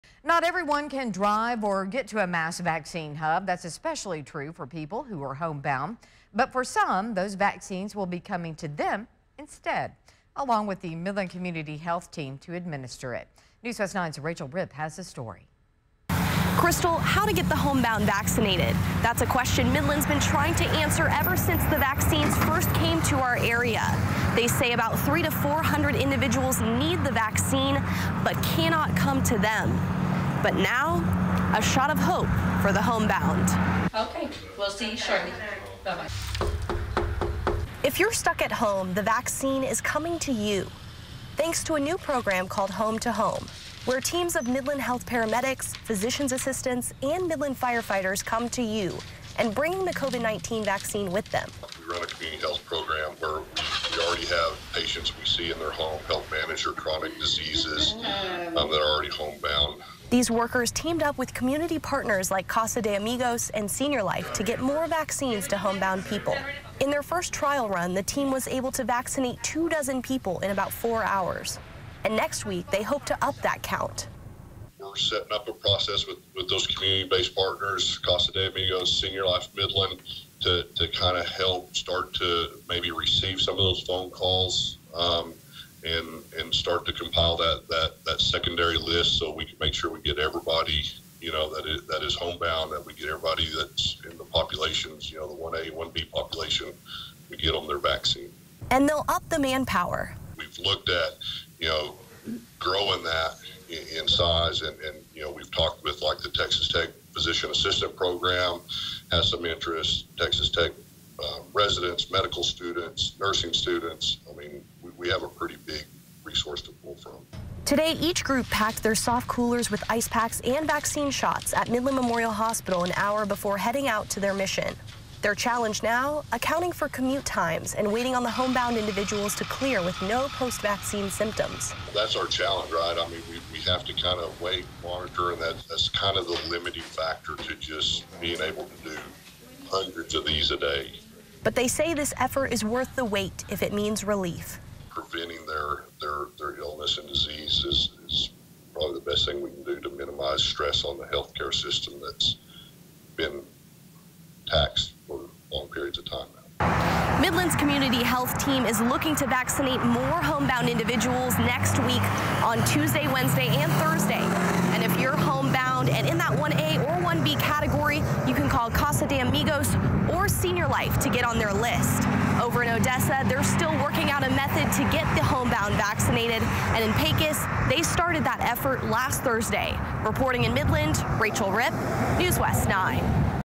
Homebound COVID-19 vaccination efforts start in Midland
Midland Health said there are about 3-400 homebound individuals in Midland.